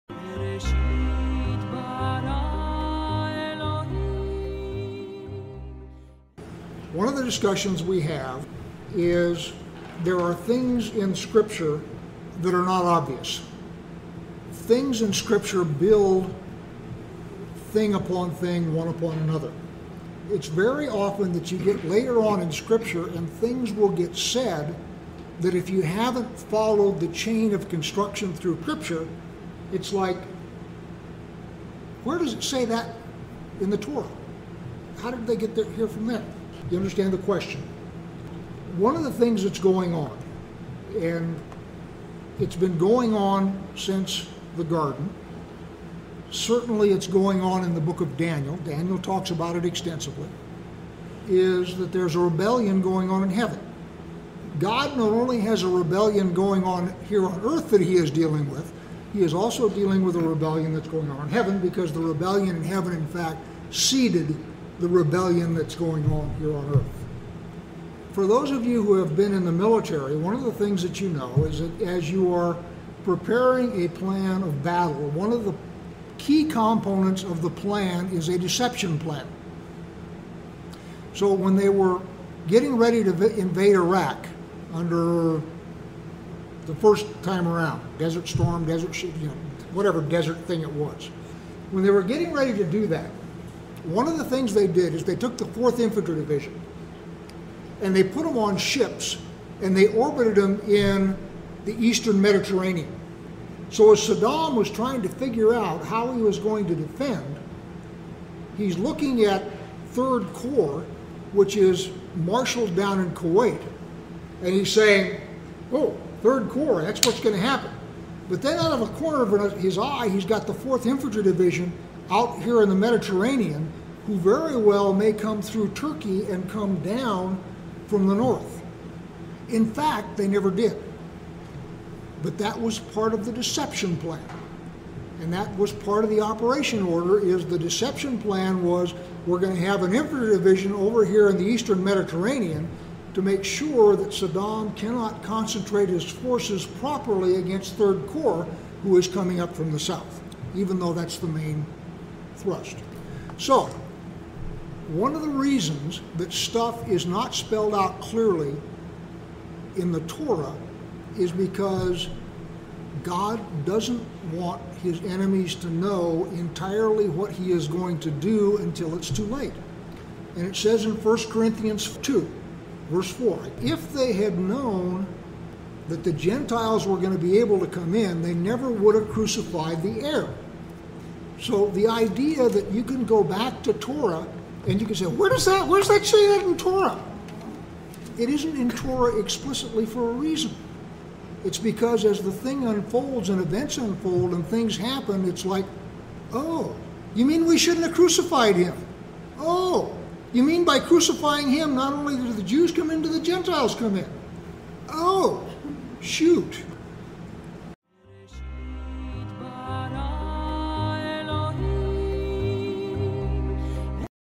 God's Deception Plan
People wonder why some things are not spelled out clearly in the Torah. It might be because God is dealing with a rebellion and doesn't want the enemy to know His plans.